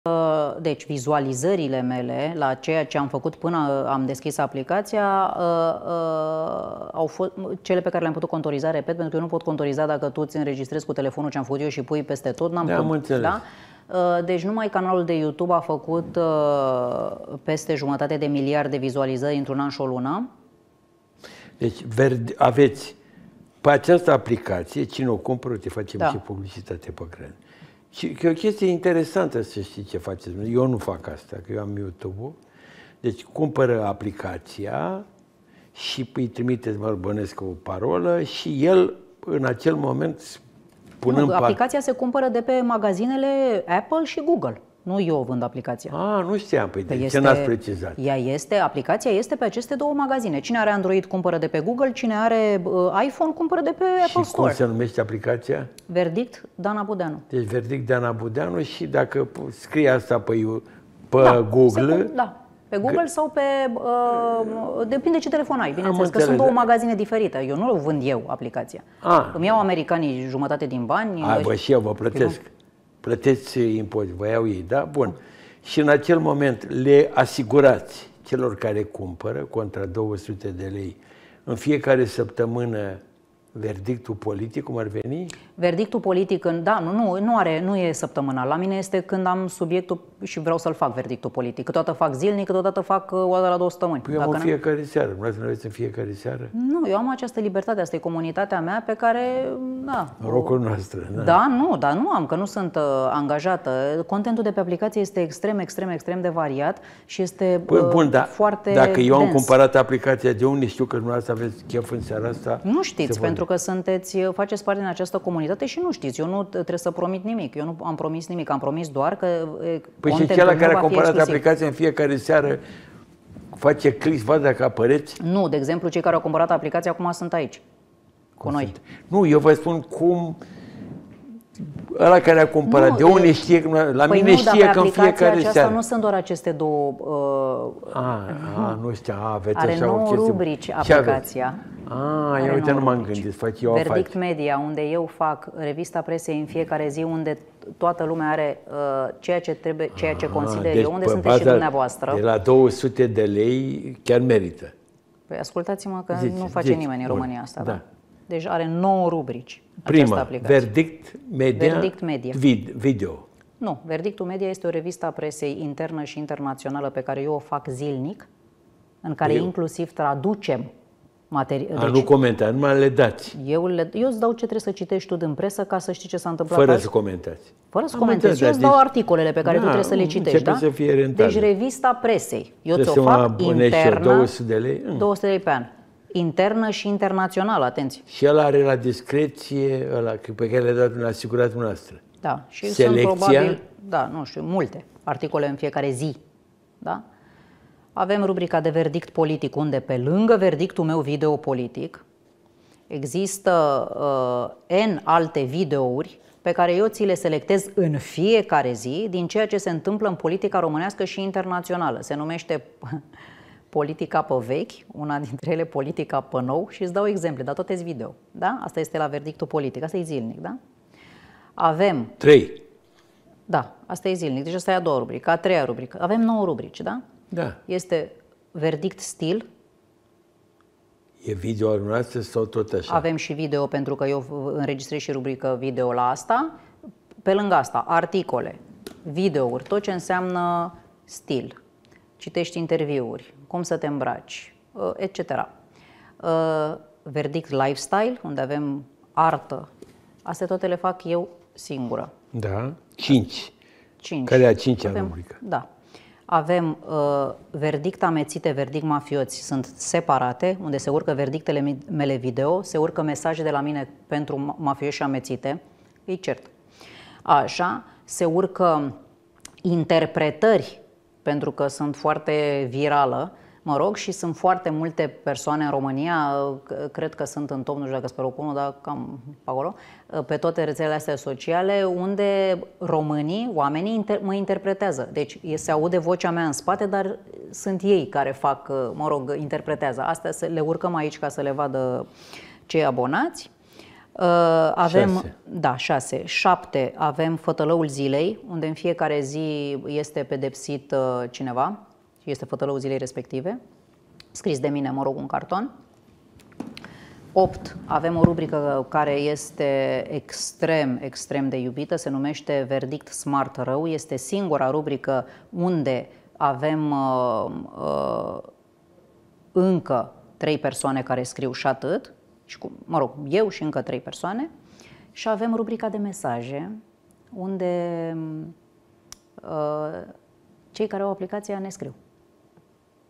Dana Budeanu: "Aplicația mea se numește Verdict Dana Budeanu, am reușit să creez o comunitate"
Față în față cu Ion Cristoiu - Dana Budeanu discută despre noua sa aplicație pentru femei.